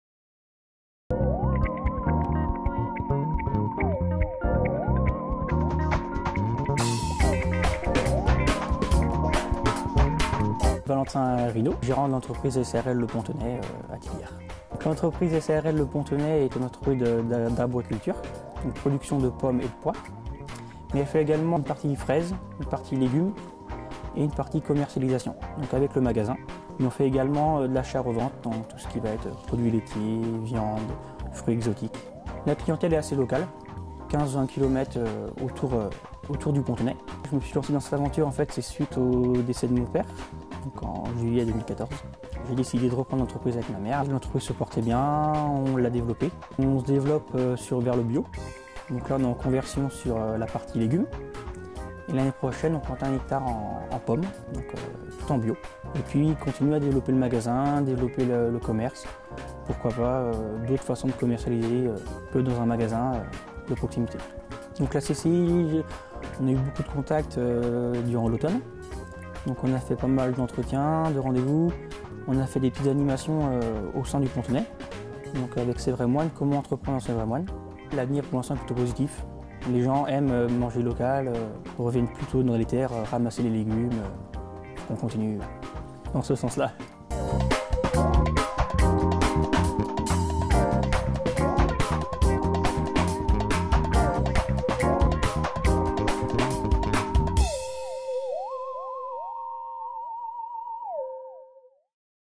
Regard sur... VERGERS DU PONTONNET
Valentin Pineau, les Vergers du Pontonnet, Tillières (production de fruits et légumes, vente directe)